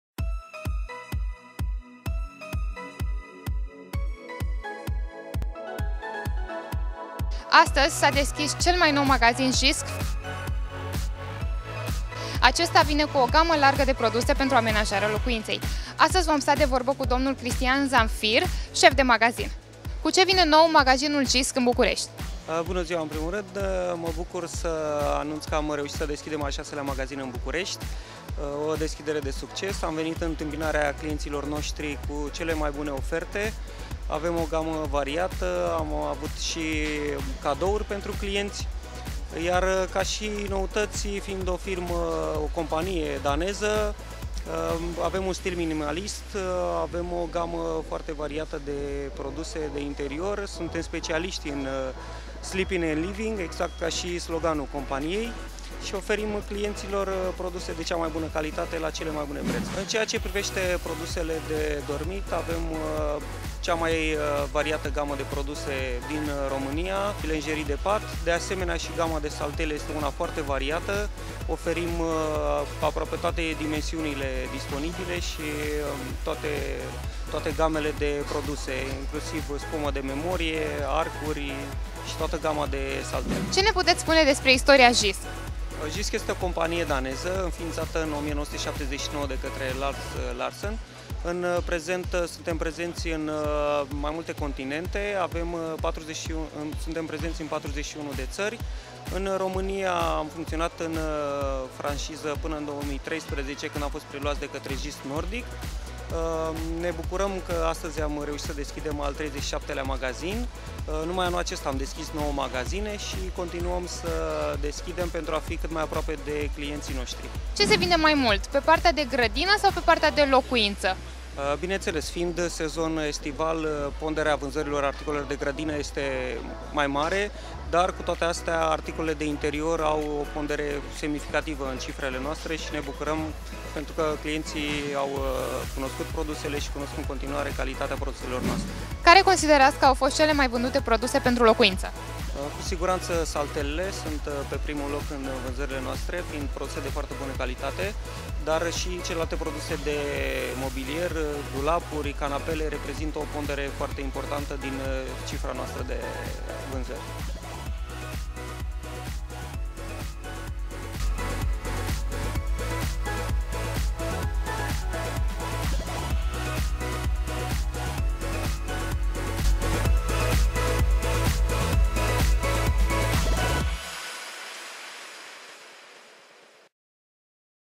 Interviu Cristian Zamfir, Store Manager JYSK
JYSK a deschis un nou magazin in Bucuresti